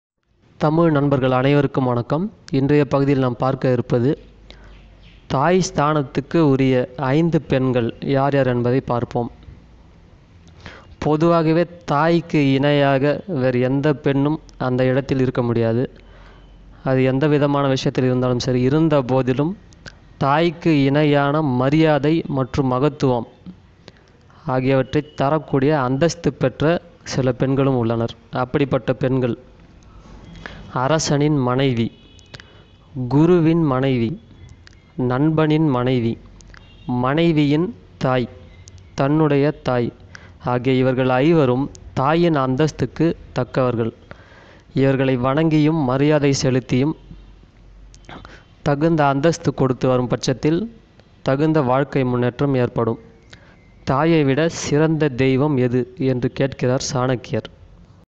சாணக்கிய நீதி
தாய் ஸ்தானத்தில் பார்க்க வேண்டிய பெண்கள்